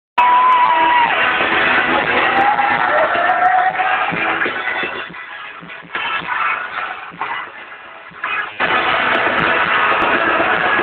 Joe Jonas Disappear on stage?
looks like joe disappeared. superpowers? super speed! he is just magical! perfection! gorgeous with super powers!. shame its not real!. i really believed he had powers until i realised after watching it a few times later that, i had just paused my phone while filming then continued!! ahahaha im soo stupid!